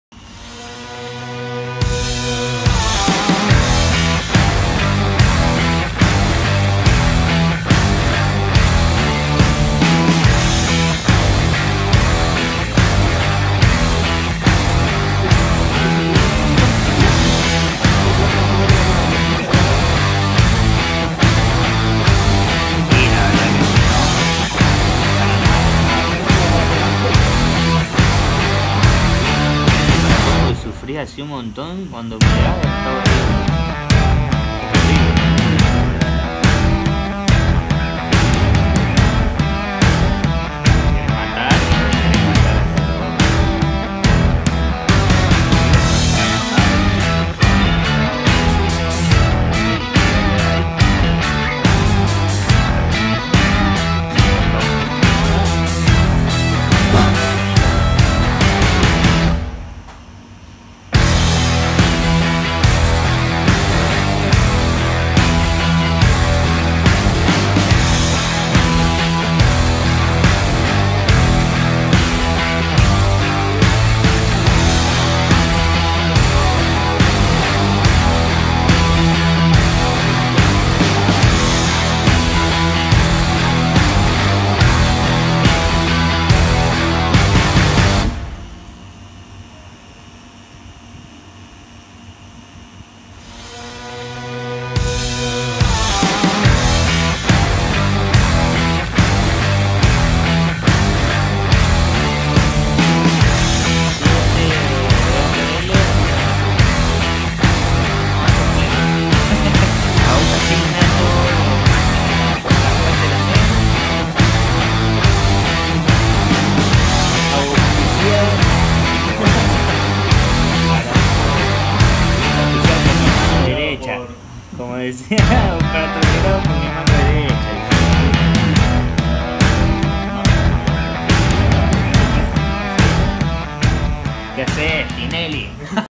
Realismo: tatuando el blanco
@pulgamarconetto tatuando el blanco en un Tattoo de realismo con @sunskintattoo Stilo Pen Machine y agujas theaignatureneedle cartucho.